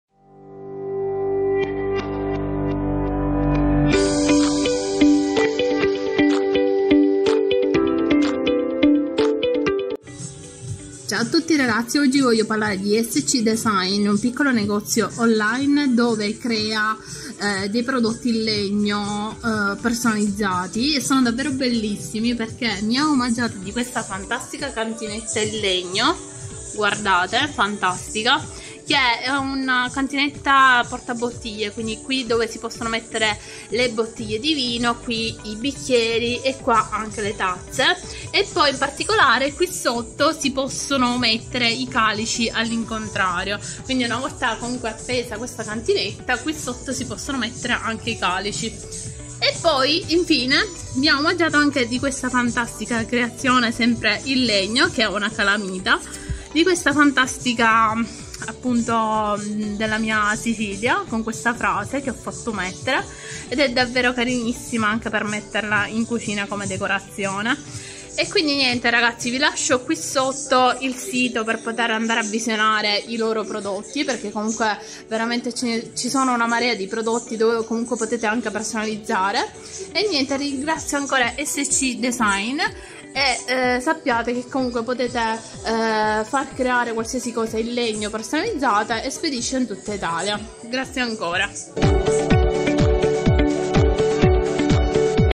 SCDESING86 - QUANDO ARTE E PASSIONE SI UNISCONO ESCONO DELLE BELLISSIME CREAZIONI IN LEGNO
Una Nuova super collaborazione

Oggi vi parlo di
Un negozio online dove realizza tantissime cose  in legno personalizzate.

È fantastica questa cantinetta portabottiglie e tazze con sotto degli inserti per i calici...
Non può mai mancare in cucina

Ma vogliamo parlare anche della Calamita a Forma di Sicilia con questa scritta che ho fatto inserire? 🤩Adoro

Grazie ancora
@scdesing86 🤩 un artista veramente